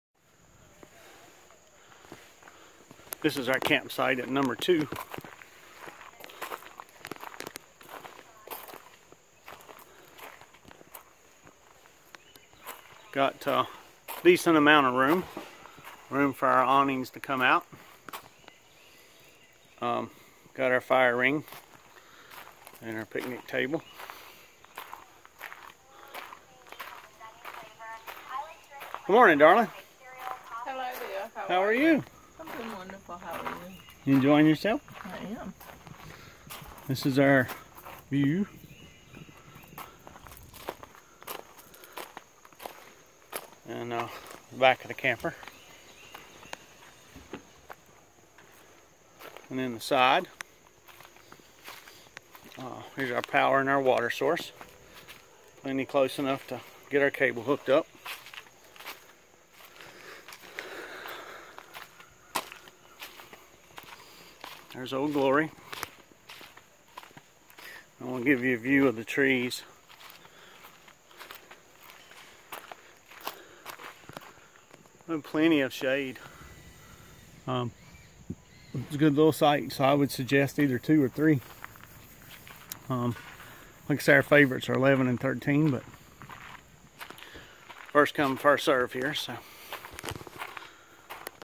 Smith Mountain Lake State Park Campsite #2
Our campsite at Smith Mountain Lake. Relaxing in the woods. Nice quite, well taken care of campground.